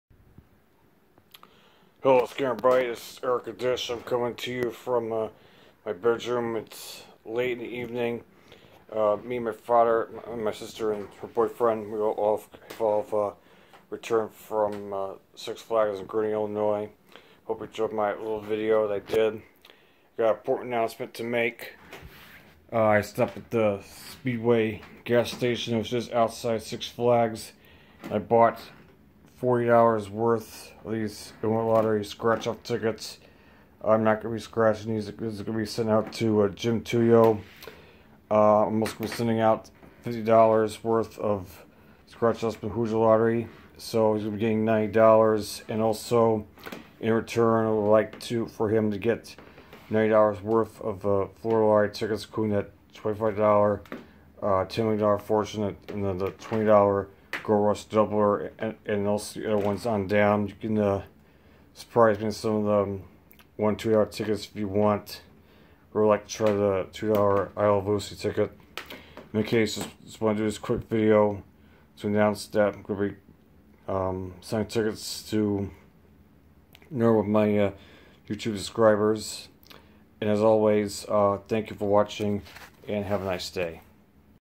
Tickets For Tuyo
A quick video in which I announce that I have bought the first set of tickets for Jim Tuyo to scratch.